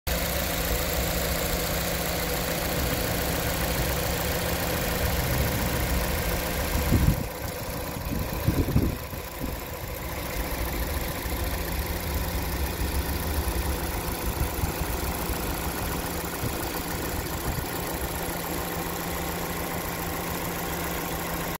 GovDeals: 2018 Ford Escape 4-DR, 2.5L L4
2018 Ford Escape 4-DR, 2.5L L4 sold by State of South Carolina on GovDeals.com